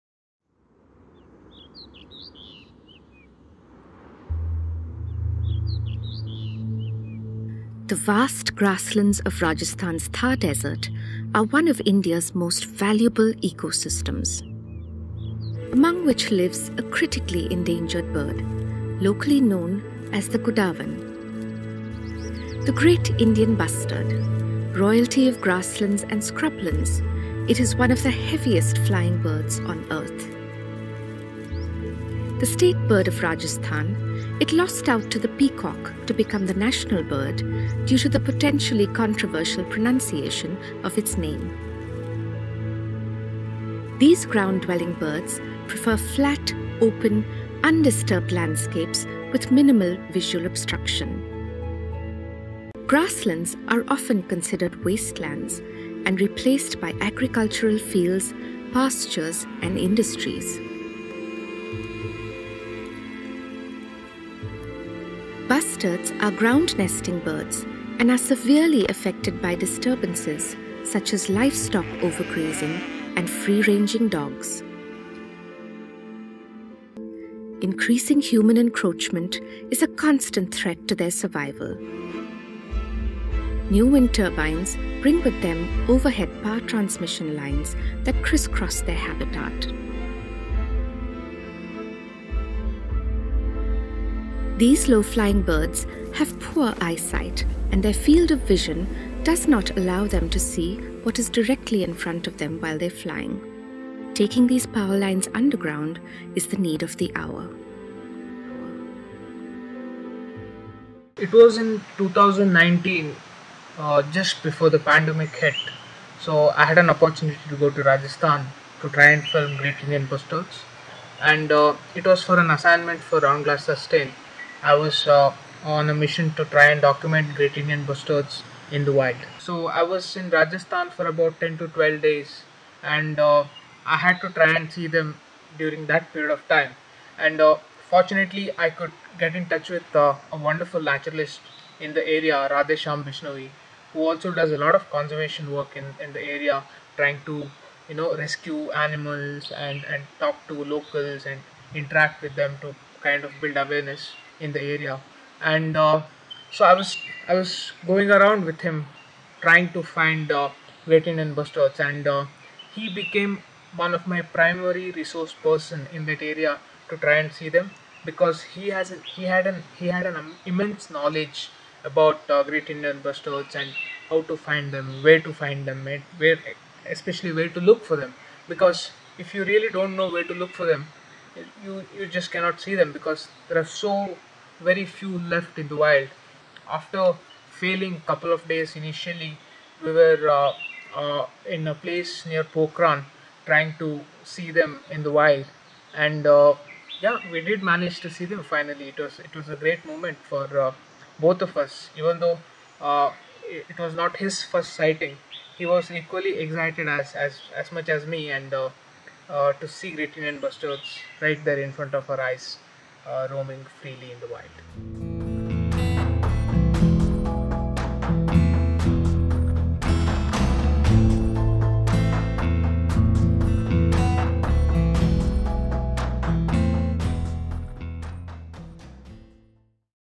Wildlife filmmaker Pradeep Hegde recalls encounters with the great Indian bustard
The great Indian bustard is a skittish and shy bird. With only about 150 left in the wild, it’s not easy to spot one. Wildlife filmmaker Pradeep Hegde went in search of the great Indian bustard in the Pokhran area of the Thar Desert, one of the large bird’s last remaining strongholds. Guided by Radheshyam Pemani Bishnoi, a local conservationist, it still took them a couple of days to catch their first sighting. Here’s a look at his adventure.